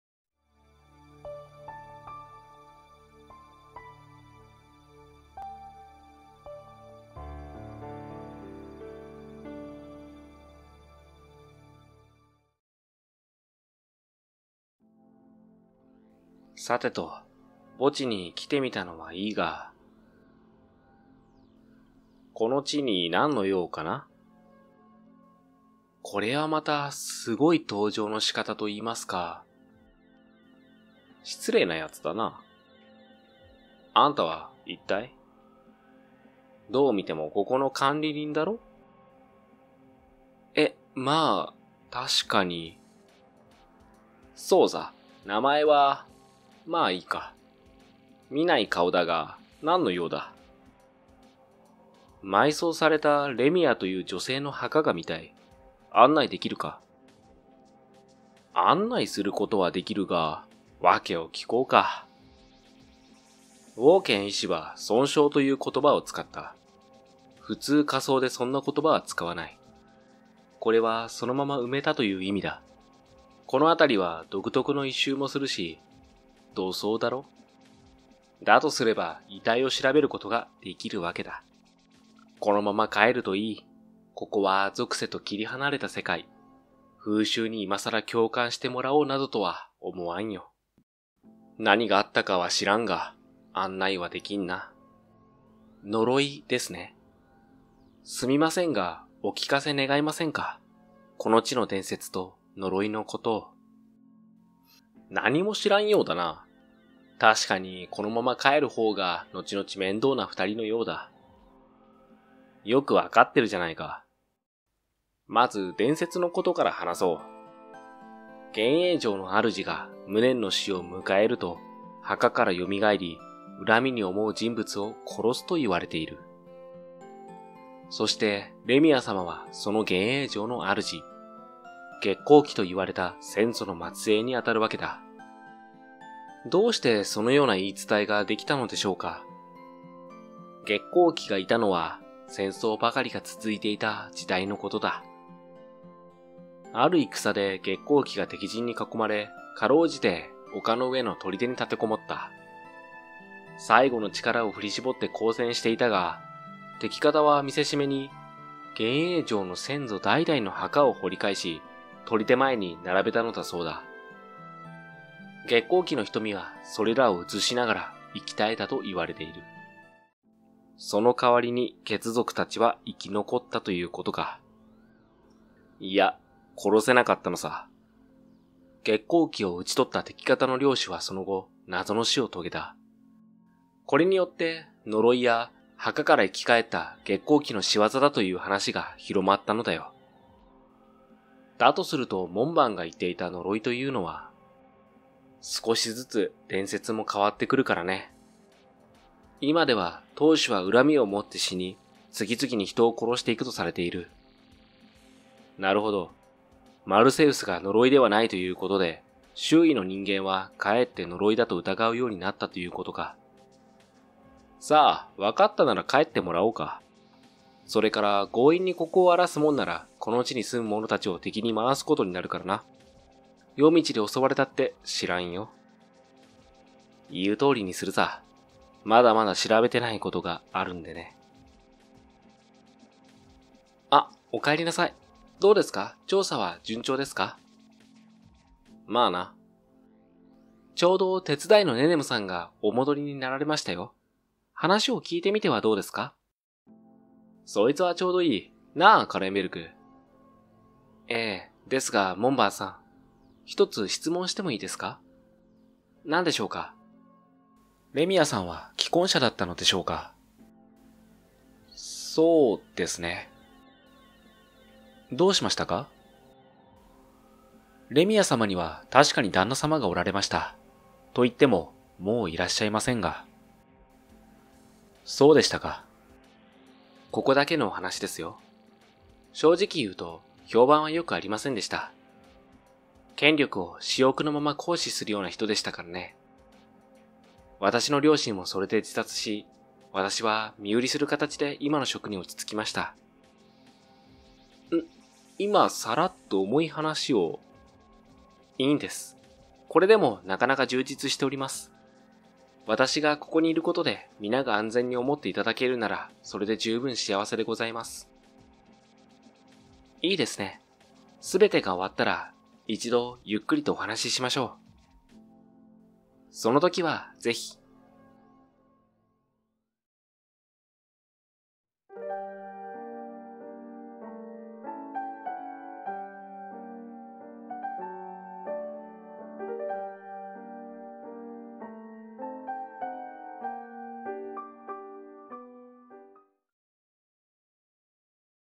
【Unlightパロ】謎解き推理本格ミステリー「第一章 呪われた城、幻影城」後半②
ー『推理は、答えを覗き見て辿り着く場所ではない』

皆様からのご意見・ご感想・ミステリー問題など募集しております。